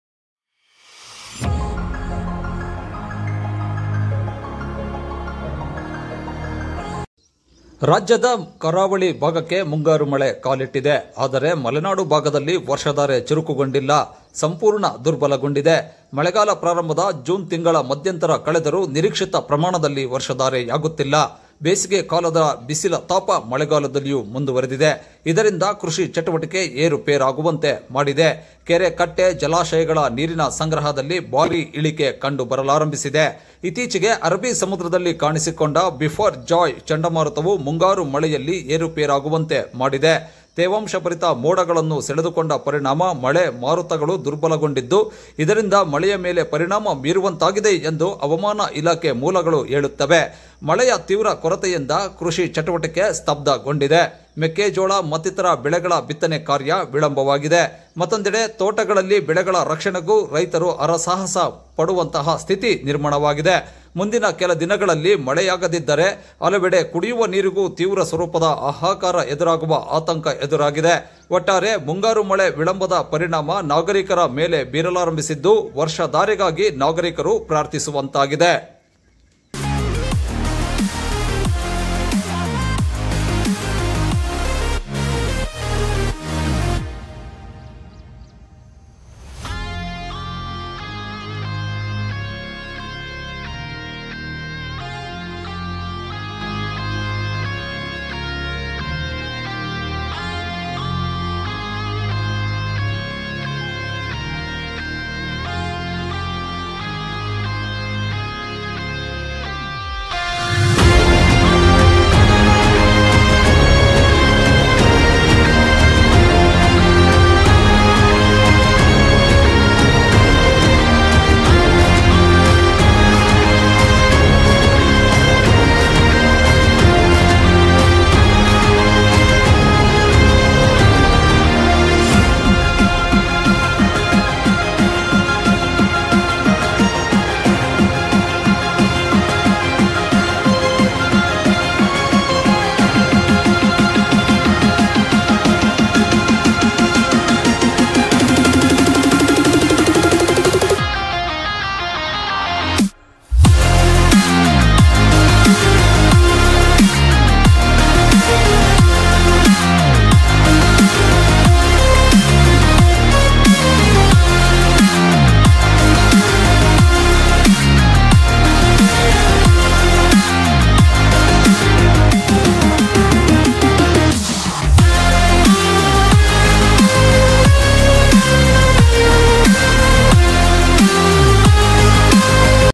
ಮುಂಗಾರು ಮಳೆ ಕಣ್ಮರೆ : ಬರಿದಾಗುತ್ತಿರುವ ಕೆರೆಕಟ್ಟೆ - ಜಲಾಶಯಗಳು! Monsoon Rain Draining Lake Reservoirs!
ಮಲೆನಾಡಿನಲ್ಲಿ ಮಳೆ ಕೊರತೆ : ಬರಿದಾಗುತ್ತಿರುವ ಕೆರೆಕಟ್ಟೆ, ಜಲಾಶಯಗಳು!

ರಾಜ್ಯದ ಕರಾವಳಿ ಭಾಗಕ್ಕೆ ಮುಂಗಾರು ಮಳೆ ಕಾಲಿಟ್ಟಿದೆ.  ಆದರೆ ಮಲೆನಾಡು ಭಾಗದಲ್ಲಿ ವರ್ಷಧಾರೆ ಚುರುಕುಗೊಂಡಿಲ್ಲ. ಸಂಪೂರ್ಣ ದುರ್ಬಲಗೊಂಡಿದೆ. ಮಳೆಗಾಲ ಪ್ರಾರಂಭದ ಜೂನ್ ತಿಂಗಳ ಮಧ್ಯಂತರ ಕಳೆದರೂ ನಿರೀಕ್ಷಿತ ಪ್ರಮಾಣದಲ್ಲಿ ವರ್ಷಧಾರೆಯಾಗುತ್ತಿಲ್ಲ. ಬೇಸಿಗೆ ಕಾಲದ ಬಿಸಿಲ ತಾಪ ಮಳೆಗಾಲದಲ್ಲಿಯೂ ಮುಂದುವರಿದಿದೆ. ಇದರಿಂದ ಕೃಷಿ ಚಟುವಟಿಕೆ ಏರುಪೇರಾಗುವಂತೆ ಮಾಡಿದೆ. ಕೆರೆಕಟ್ಟೆ, ಜಲಾಶಯಗಳ ನೀರಿನ ಸಂಗ್ರಹದಲ್ಲಿ ಭಾರೀ ಇಳಿಕೆ ಕಂಡುಬರಲಾರಂಭಿಸಿದೆ. ಇತ್ತೀಚೆಗೆ ಅರಬ್ಬಿ ಸಮುದ್ರದಲ್ಲಿ ಕಾಣಿಸಿಕೊಂಡ ಬಿಪರ್ ಜಾಯ್ ಚಂಡಮಾರುತವು ಮುಂಗಾರು ಮಳೆಯಲ್ಲಿ ಏರುಪೇರಾಗುವಂತೆ ಮಾಡಿದೆ. ತೇವಾಂಶಭರಿತ ಮೋಡಗಳನ್ನು ಸೆಳೆದುಕೊಂಡ ಪರಿಣಾಮ ಮಳೆ ಮಾರುತಗಳು ದುರ್ಬಲಗೊಂಡಿದ್ದು, ಇದರಿಂದ ಮಳೆಯ ಮೇಲೆ ಪರಿಣಾಮ ಬೀರುವಂತಾಗಿದೆ ಎಂದು ಹವಾಮಾನ ಇಲಾಖೆ ಮೂಲಗಳು ಹೇಳುತ್ತವೆ.
ಮಳೆಯ ತೀವ್ರ ಕೊರತೆಯಿಂದ ಕೃಷಿ ಚಟುವಟಿಕೆ ಸ್ತಬ್ದಗೊಂಡಿದೆ. ಮೆಕ್ಕೆಜೋಳ ಮತ್ತೀತರ ಬೆಳೆಗಳ ಬಿತ್ತನೆ ಕಾರ್ಯ ವಿಳಂಬವಾಗಿದೆ. ಮತ್ತೊಂದೆಡೆ, ತೋಟಗಳಲ್ಲಿ ಬೆಳೆಗಳ ರಕ್ಷಣೆಗೂ ರೈತರು ಹರಸಾಹಸ ಪಡುವಂತಹ ಸ್ಥಿತಿ ನಿರ್ಮಾಣವಾಗಿದೆ.

OverturnedTanker ಟ್ಯಾಂಕರ್ ಪಲ್ಟಿ : ತಪ್ಪಿದ ಭಾರೀ ದುರಂತ !